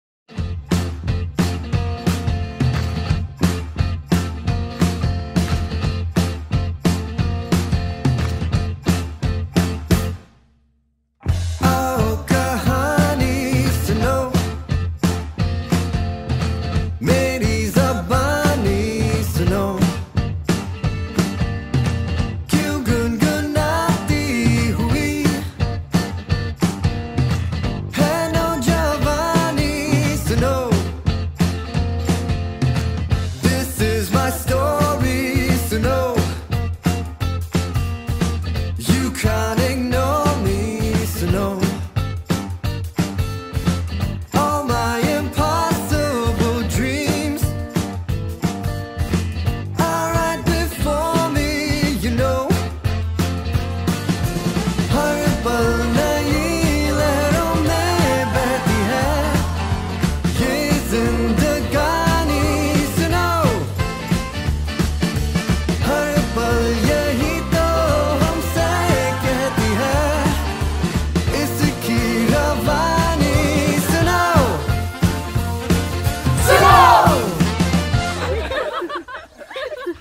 Promo Shoot: The Archies Rendition | Tenth Floor Mediaworks
This is an experimental approach to showcase our side of 'The Archies' movie. This is a low budget rendition but we have tried to incorporate film looks for this video. The video was shot on Sony A7c.